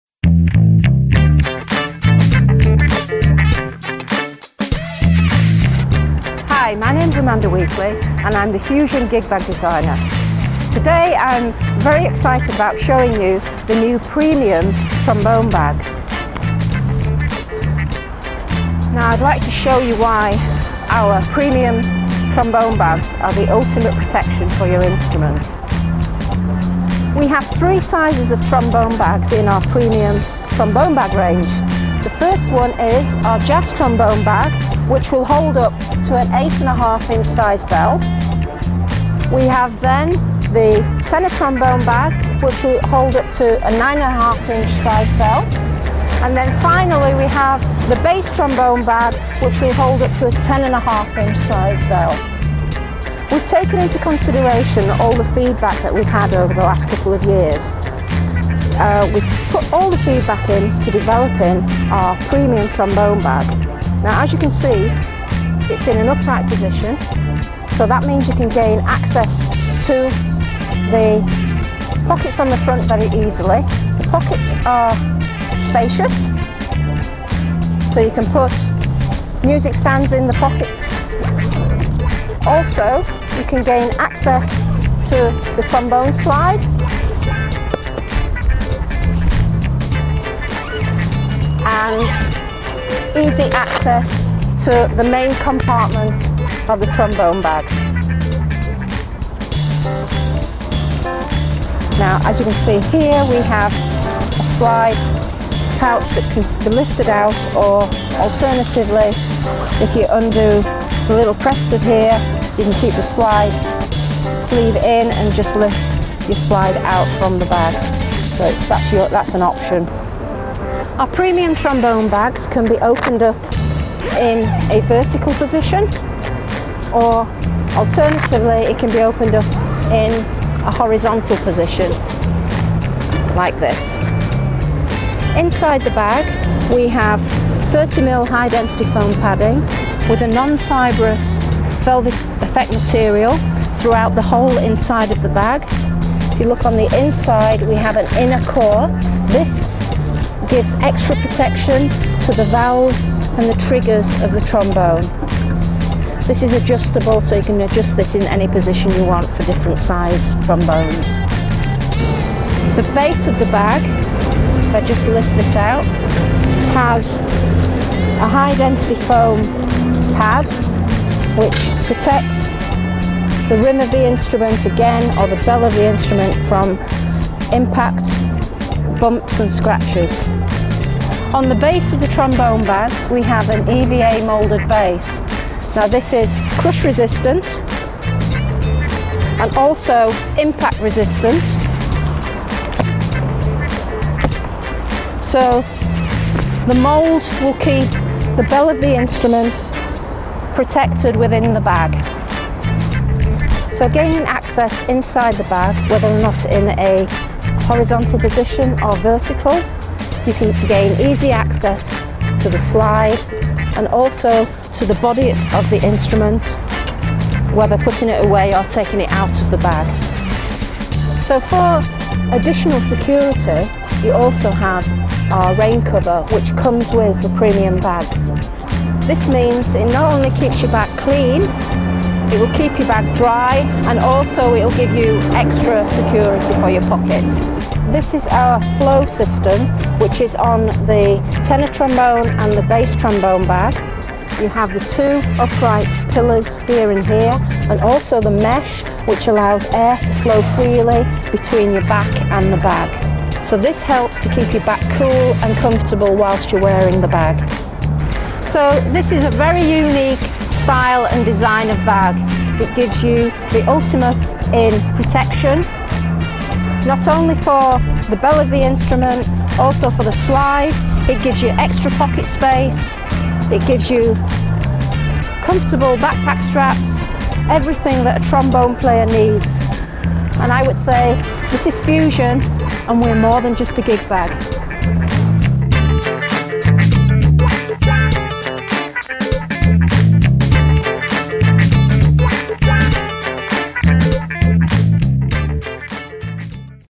Premium Trombone Gig Bags (by Fusion-Bags.com)
The Premium 8.5", Jazz Trombone, 9.5" Tenor Trombone and 10.5" Bass Trombone gig bag is the perfect bag for a musician who needs to carry their instrument without compromising its safety. The new design allows you to fully open the main compartment of the bag and access your trombone with ease.

Tough on the outside
Constructed with robust non PVC backed water resistant rip-stop material, the Fusion Premium 9.5" Tenor Trombone offers extreme resistance to scuffs and abrasions. With soft rubber handles and zip pulls, moulded base and a hardwearing finish, the Fusion Premium gig bag is designed to last. These gig bags are available in four colours with commuter friendly reflective panels and print, a safety factor if you are a cyclist or pedestrian. The rear of the bag boasts Fusion's own "Flow-System" similar to that of a hiker's backpack, which makes the bag really comfortable to carry and helps prevent you from getting hot when it's on your back. At the rear of the "Flow-System" is a zipped compartment where the backpack straps can be stored out of sight if the bag is carried by hand. There are added features such as lockable zips on the main compartment of the bag, address tag and a rain-cover to help keep it dry in wet weather conditions.